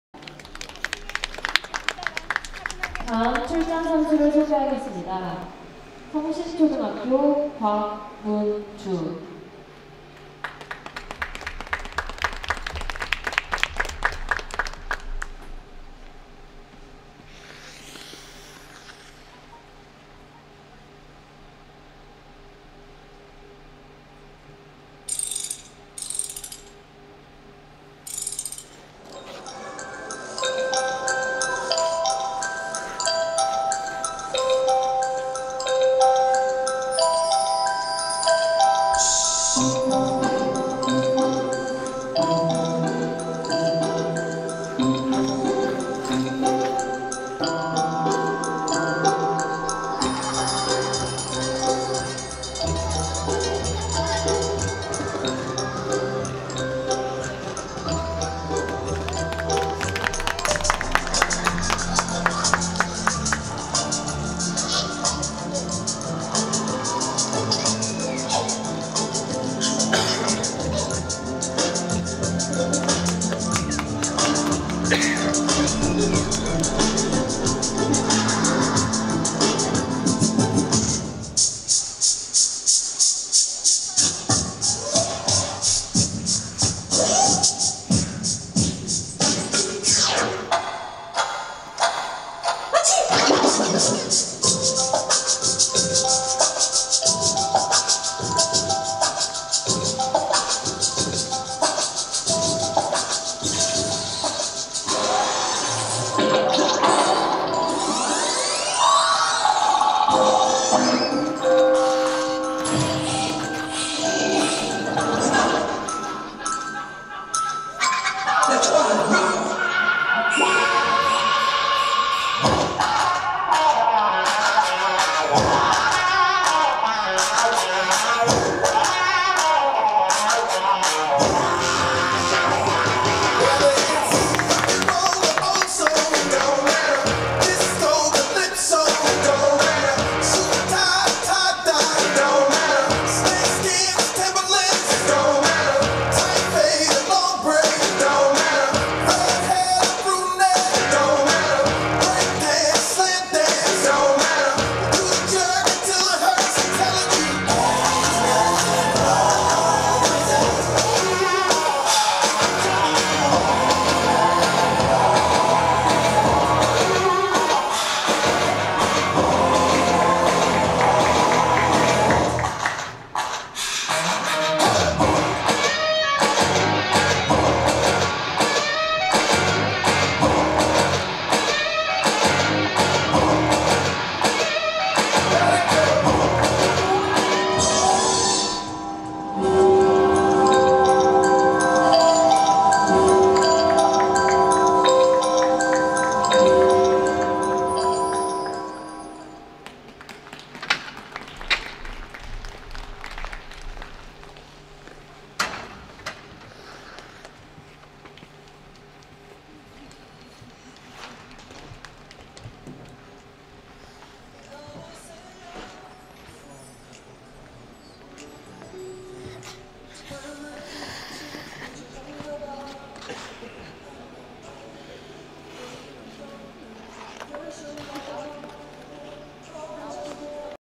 곽문주,성신초6 @ 2018년 피겨 동계체전 B조 여초부 SP #02
2018/01/13, 태릉 실내빙상장, 2018년 제99회 전국동계체전 피겨스케이팅 1일차